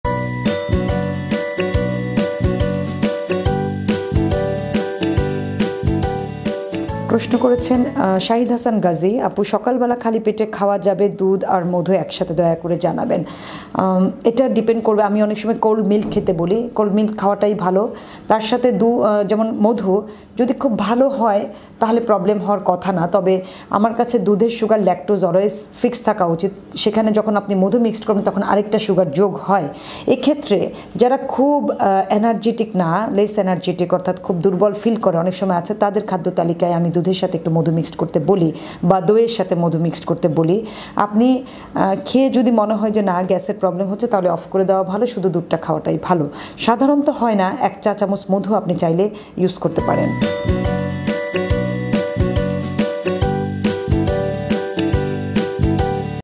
সকালে খালি পেটে দুধ আর মধু একসাথে খাওয়া যাবে? Nutritionist Aysha Siddika | Shad o Shastho
জেনে নিন পুষ্টিবিদ আয়শা সিদ্দিকার কাছ থেকে।

চেম্বার
ধানমন্ডি